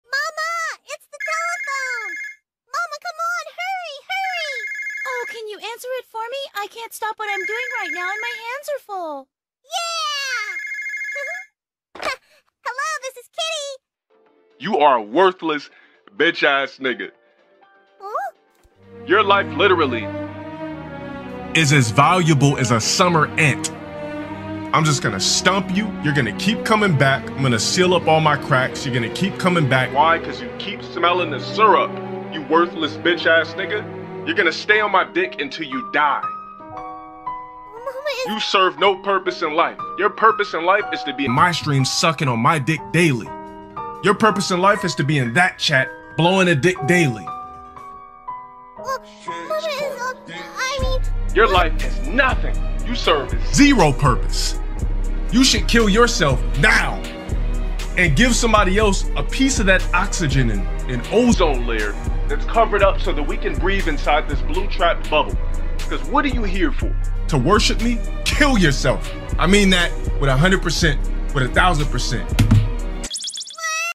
LowTierGod Calls Kitty
I was watching TikTok and I saw a Hello Kitty meme. So I made this.

Meme made by Pokeshek